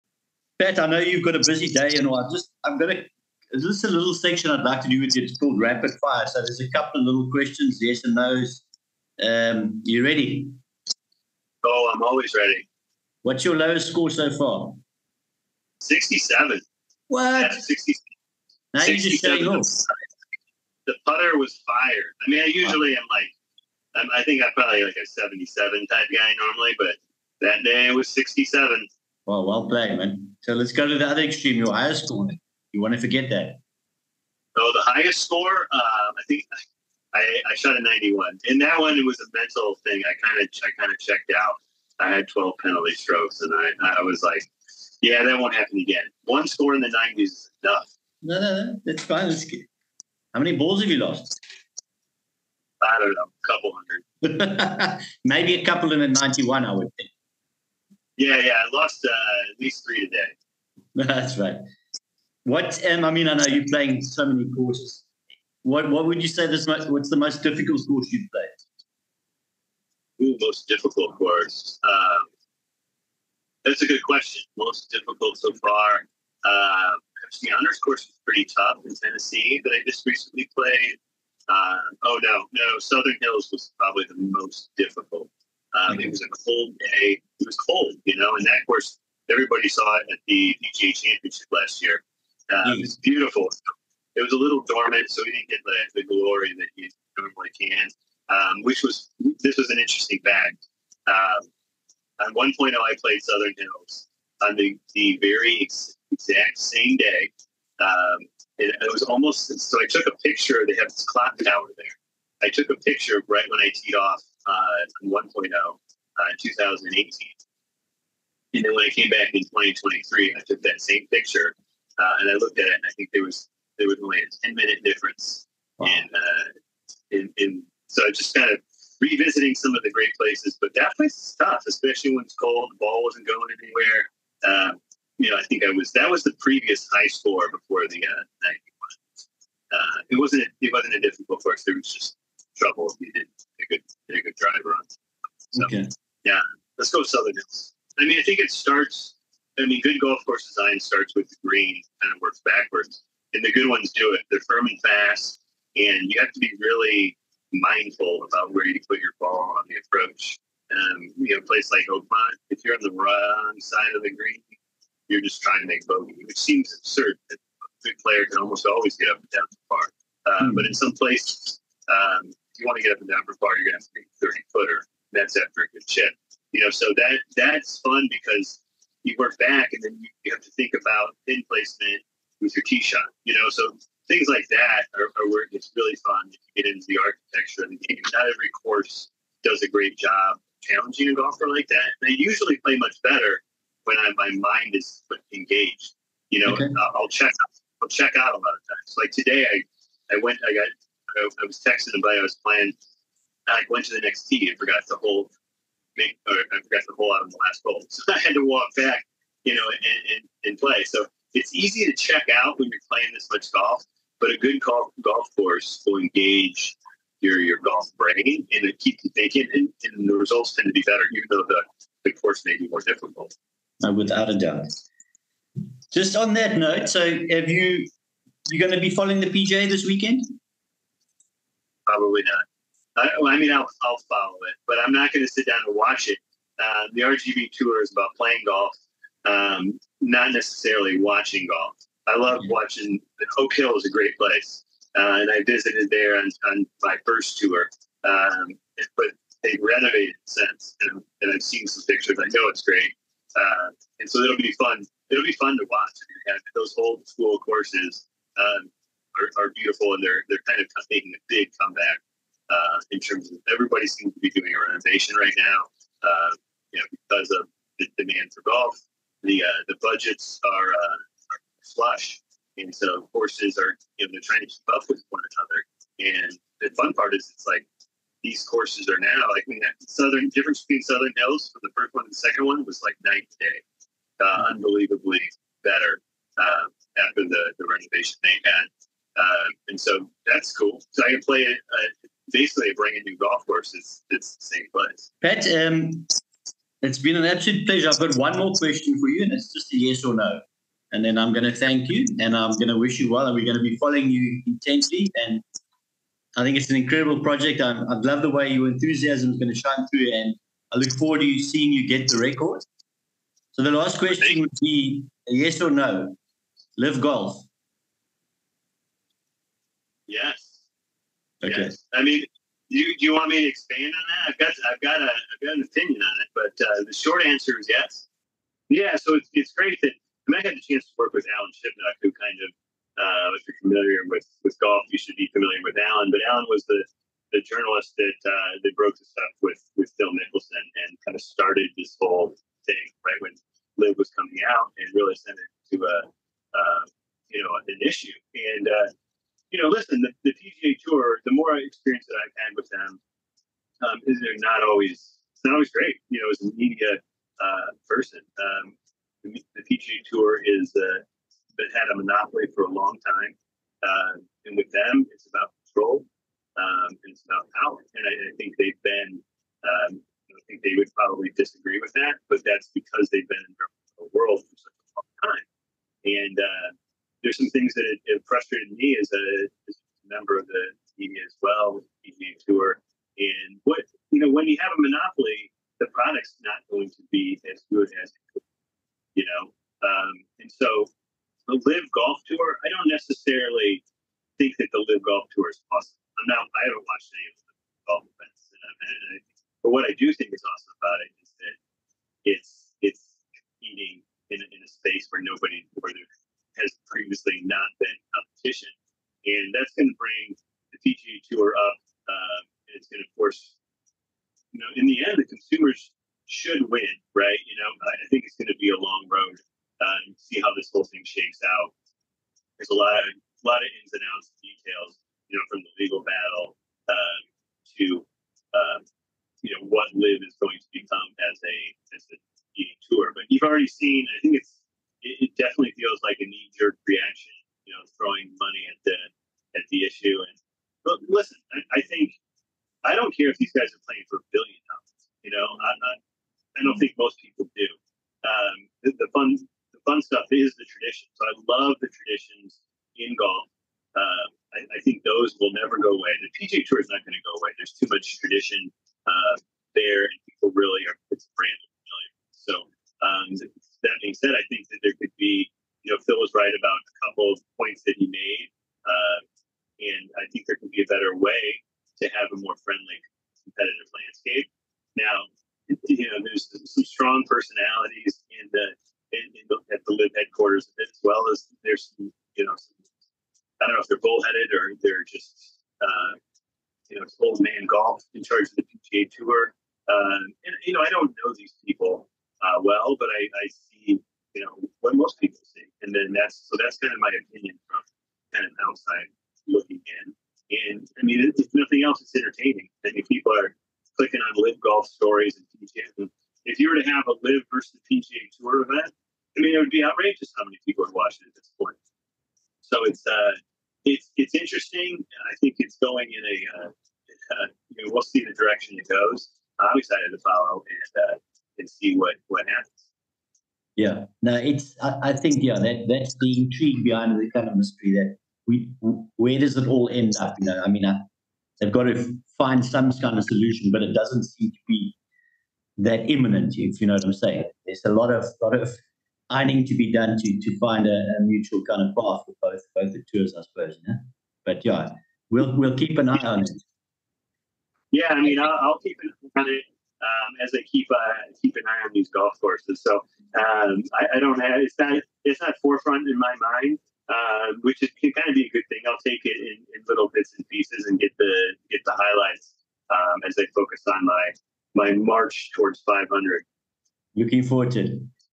RGV 2.0 I Patrick Koenig I World Record Attempt
Duck Hook Debates I Episode 5 featuring Golf photographer Patrick Koenig and his attempt to beat the World Record of the most 18 hole rounds of golf played in one year on his RGV 2.0 Tour.

Patrick has partnered with Golf GameBook to bring you The RGV Tour 2.0. As the #1 digital scorecard and social golf app, I will record all of my rounds with Golf Gamebook and upload exclusive social content.

Once again, I will be raising money for the First Tee of Greater Seattle as well as various other charities around the nation.

Follow the Time Stamp below to track your favourite question I ask Patrick

00:00 Rapid Fire Questions
00:13 Lowest score on the RGV 2.0
00:31 Highest score on the RGV 2.0
00:50 How many golf balls have you lost on the RGV 2.0
01:02 The most difficult course on the RGV 2.0
03:36 PGA Championship at Oak Hill
05:56 Patrick’s views on LIV Golf

Special Thanks to Patrick

Special Thanks African Experience offers safari, golf & day tours customised & privately hosted throughout South Africa.